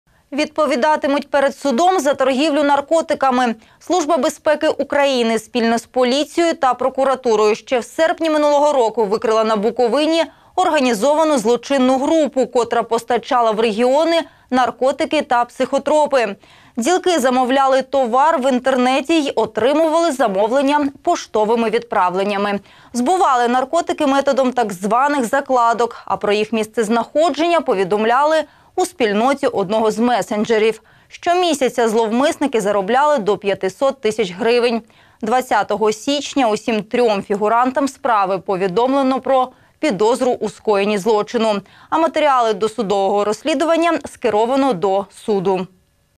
Відповідатимуть перед судом за торгівлю наркотиками
Служба безпеки України спільно з поліцією та прокуратурою, ще в серпні минулого року викрила на Буковині організовану злочинну групу, котра постачала в регіоні наркотики та психотропи.